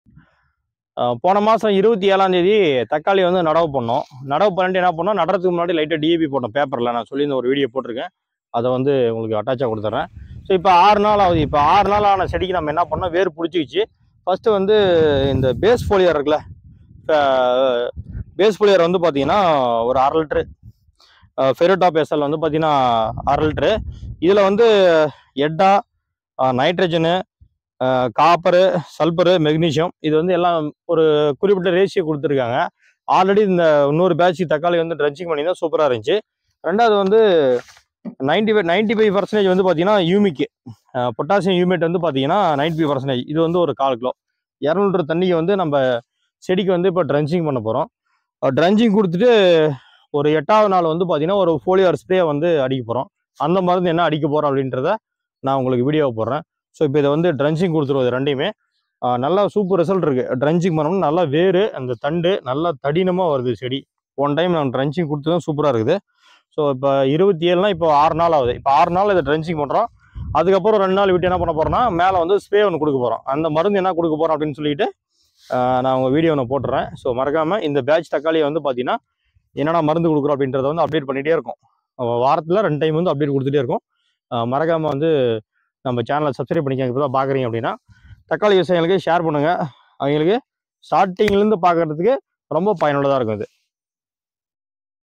முதல் உரம் தக்காளிக்கு Fertilizer for 6 Days old crop
முதல் உரம் நடவு செய்த ஆறாம் நாளில் இதை கொடுத்து உள்ளோம் மேலும் அடுத்து அடுத்து உரம் மற்றும் வளர்ச்சி மேலாண்மைக்கு பின் தொடருங்கள்